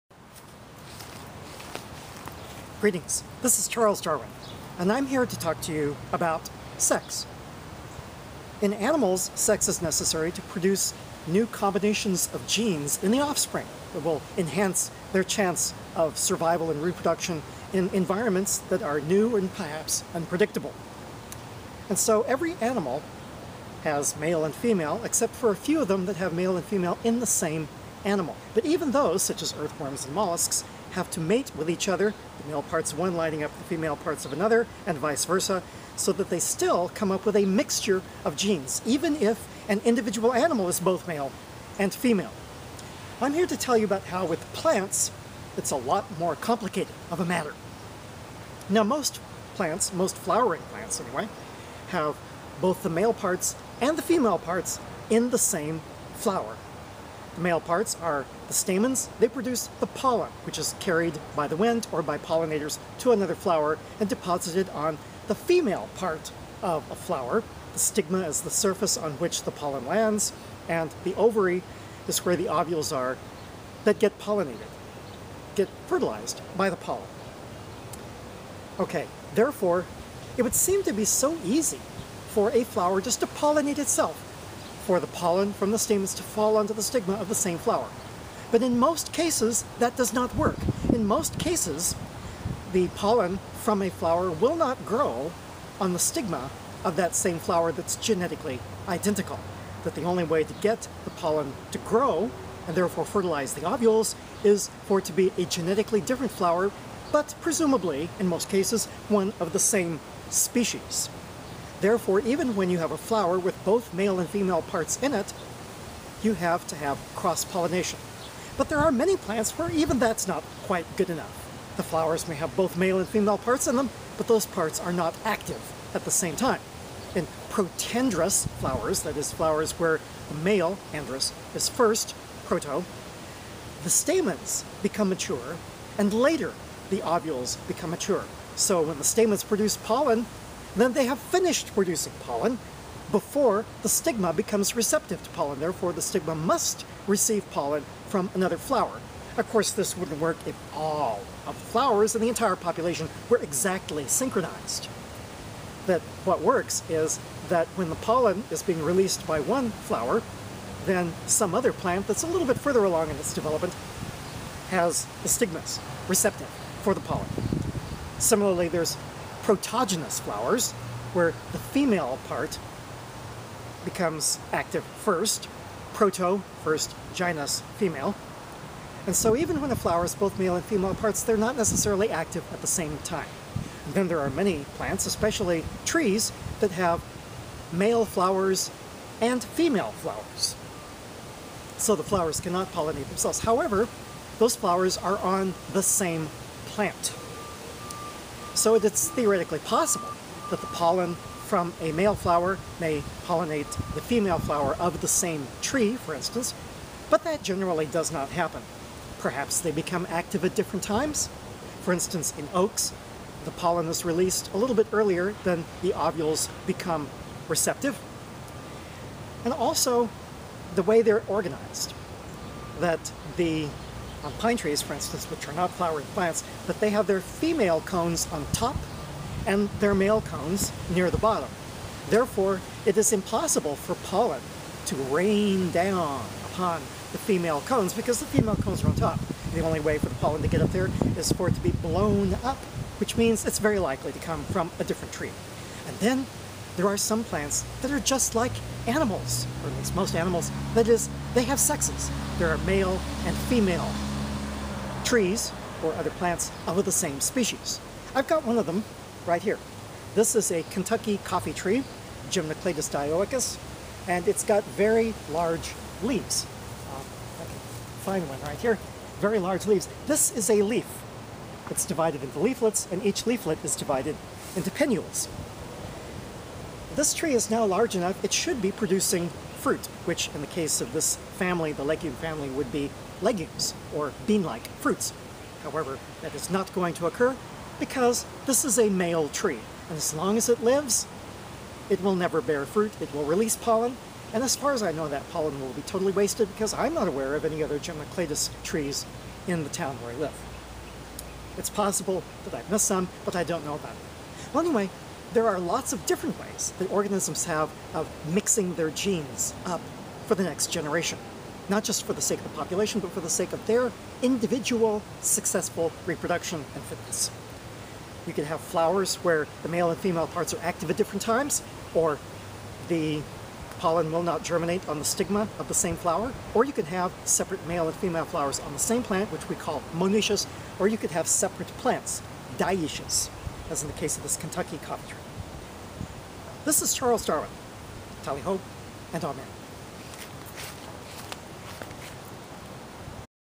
Plants have sex!
Darwin describes the many ways that plants have sexual reproduction.